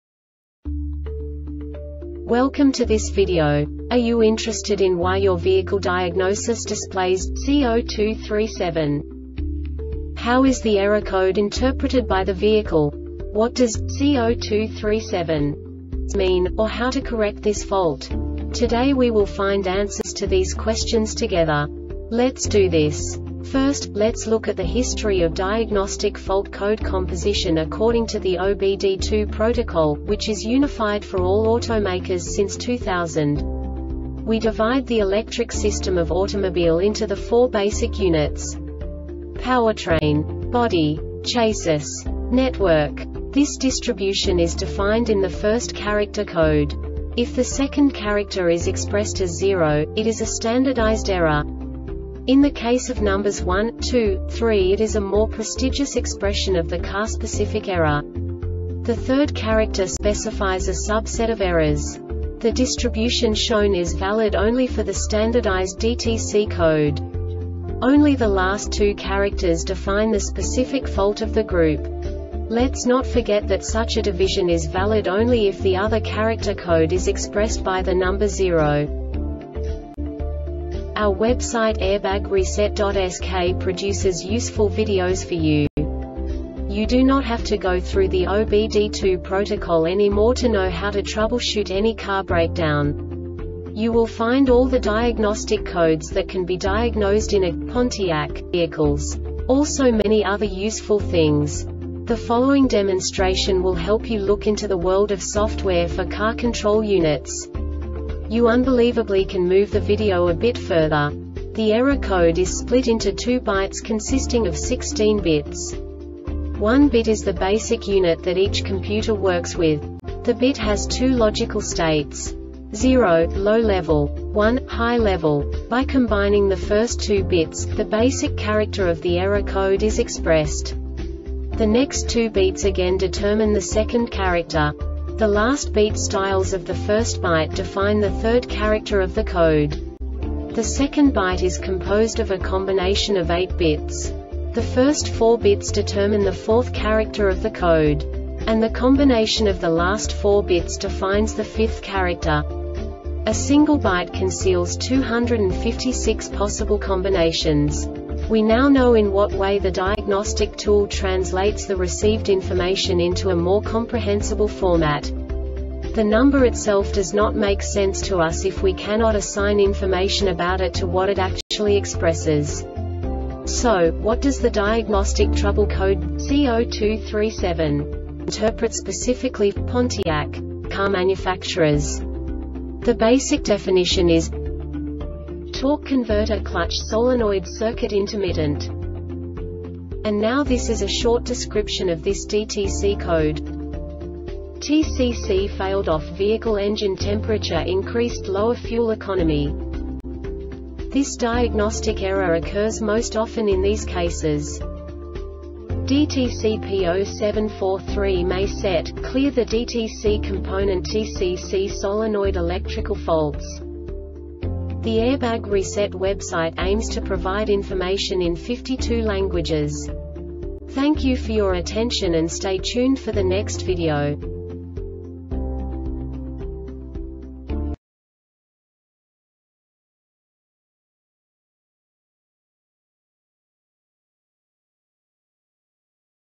DTC Pontiac C0237 Short Explanation
Contents:
0:21 Basic DTC analysis according to OBD2 protocol standard.
1:48 Insight into programming
2:58 A diagnostically understandable description of the Diagnostic Trouble Code
3:05 Basic error definition

Make:
Pontiac
Code:
C0237
Definition:
Torque Converter Clutch Solenoid Circuit Intermittent
Description:
TCC failed off Vehicle engine temperature increased Lower fuel economy
Diagnosis:
DTC P0743 may set, CLEAR the DTC .
Component:
TCC solenoid electrical faults